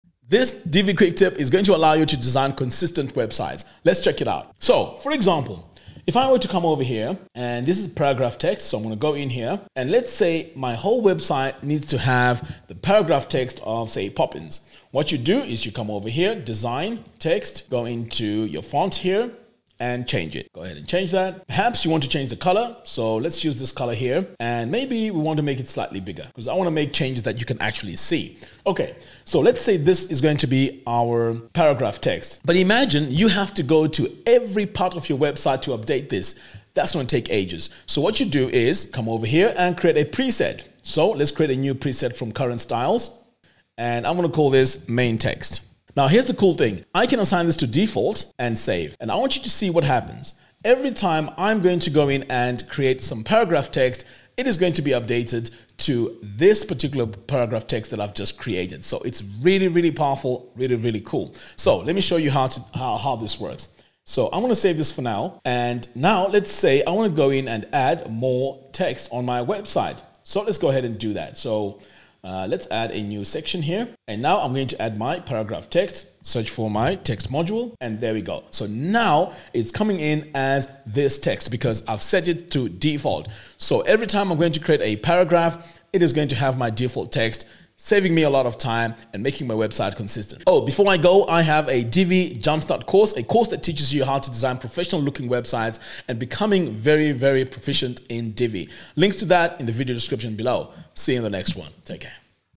Divi Theme Builder - Divi Presets Tip
In this video, I show you some Divi Theme Builder Tips. Let's take a look at Divi Presets.